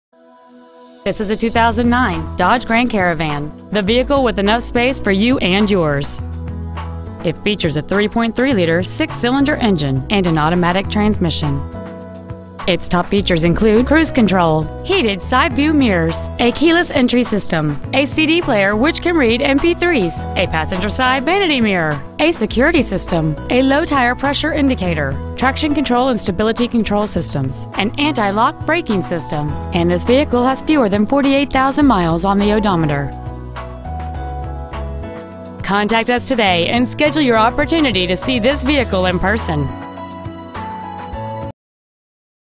2009 Dodge Grand Caravan IL
Year : 2009
 Make : Dodge
 Model : Grand Caravan
 Engine : 3.3L V6
 Trans . : Automatic
 Exterior : Maroon
 Miles : 47,620

 Grossinger Autoplex
 6900 N. McCormick Blvd
  , IL 60712